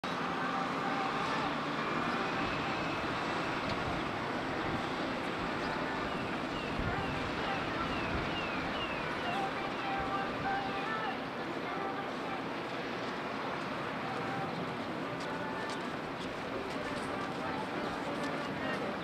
Несанкционированный митинг 2021 в Набережных челнах власть хорошо подготовилась сегодня
КлассическийПодход/ClassicApproach Чип и Дейл спешат на помощь